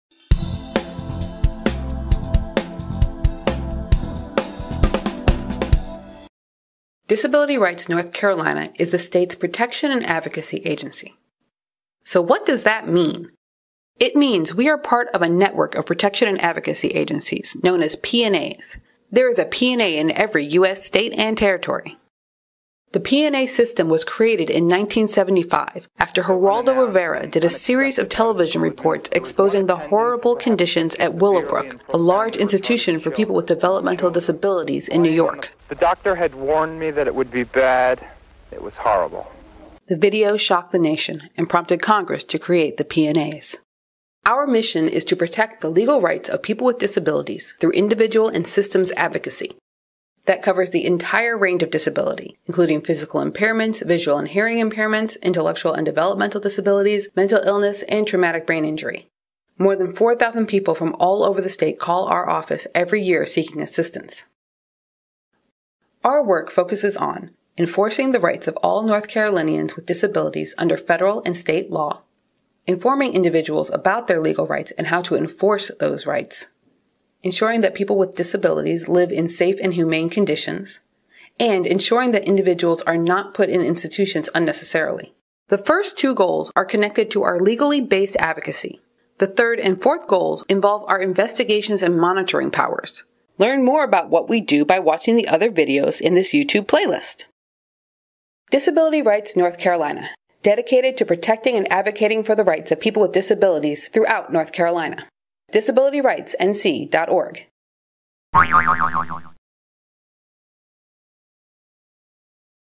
About Disability Rights NC
Disability Rights NC is the state's protection and advocacy agency. Our mission is the protect the legal rights of people with disabilities through individual and systems advocacy.